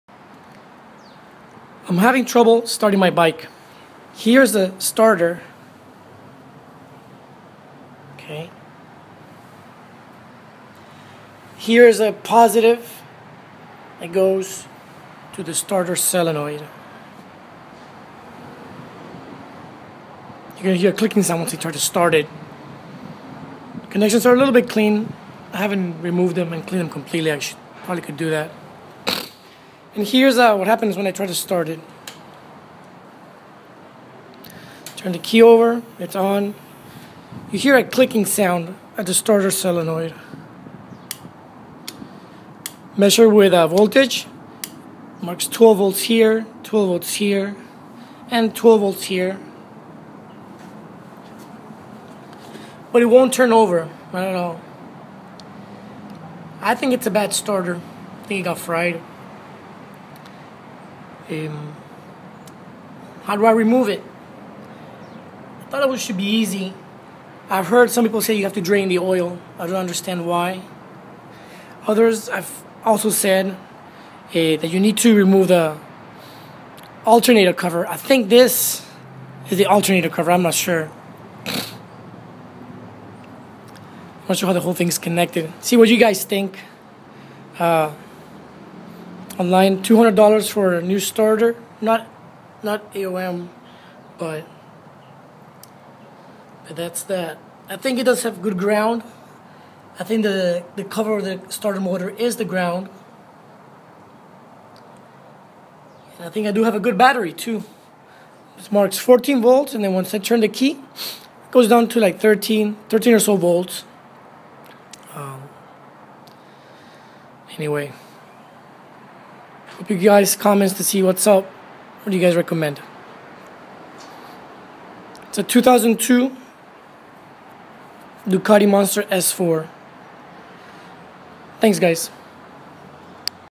Starter motor
Motorcycle will not turn over. Get 14 V on battery, when turn switch on, read 13 V. At the starter lead +, and the starter solenoid, read 12V. Tried start pushing he bike and did not start.

Before it stopped starting, I have having trouble starting it cold. It would take 3 to 5 attempts. Once warm it would start right up.

I believe the starter motor is fried, so want advice on how to replace it. Read that need drain oil. Why? Also need remove the alternator fan cover. Is it the one pointed in the video?

If fried, why did it fried? From too much current for too long when starting it cold? Why was it not starting right up when cold?